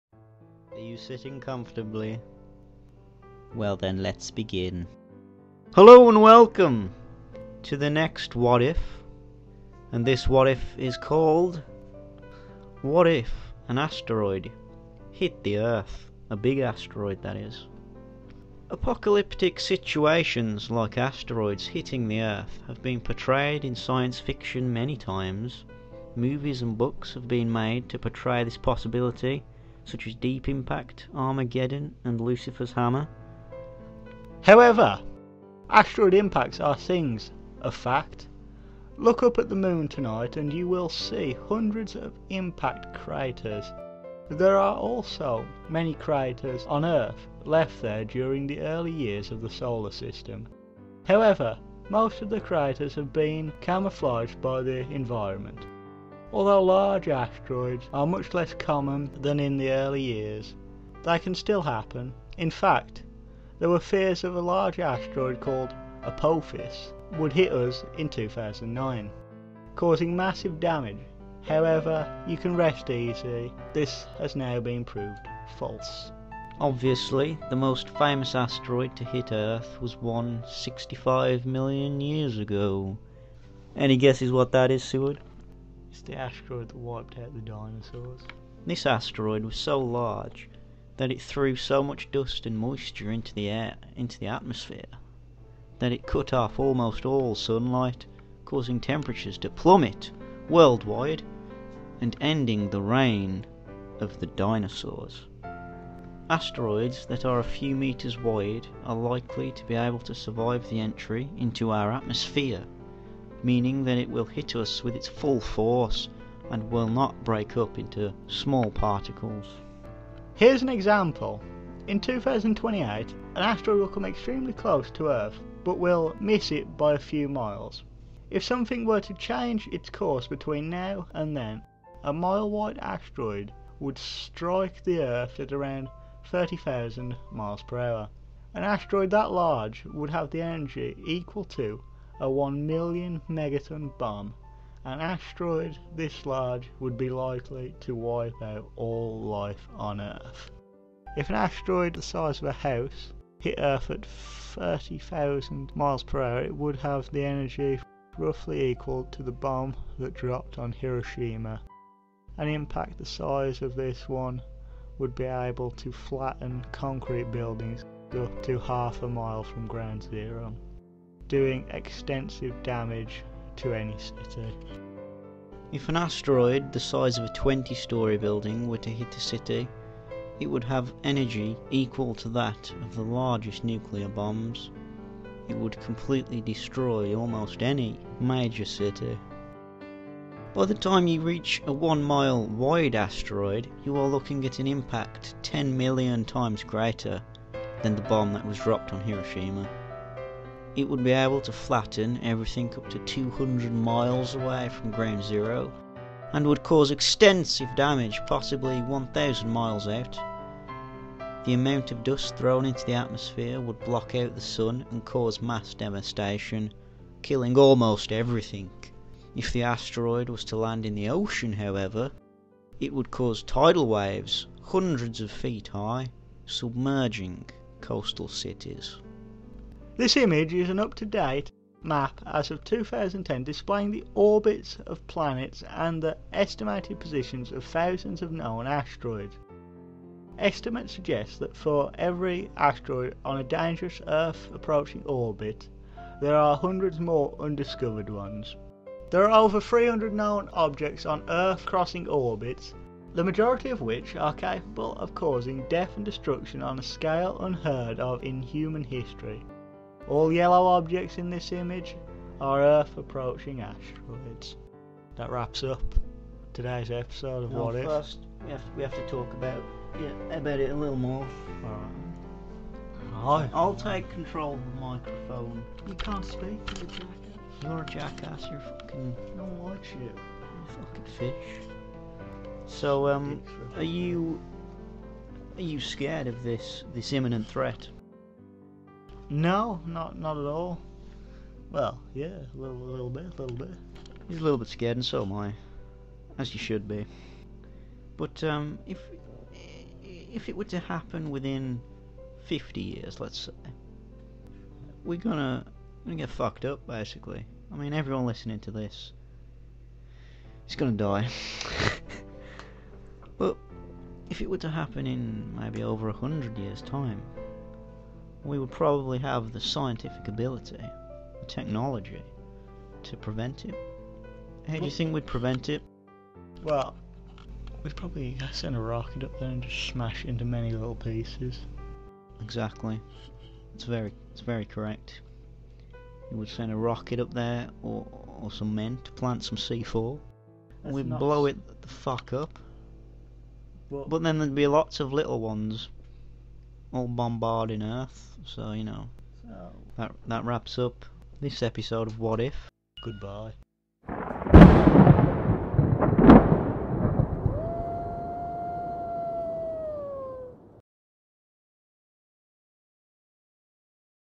WHAT IF A Massive Asteroid hit the Earth?
● An in-depth and highly intelligent description on what would happen if a massive asteroid hit the Earth ● In this video we also reveal the little known fact of how the dinosaurs were extinct ● Featuring Aphophis.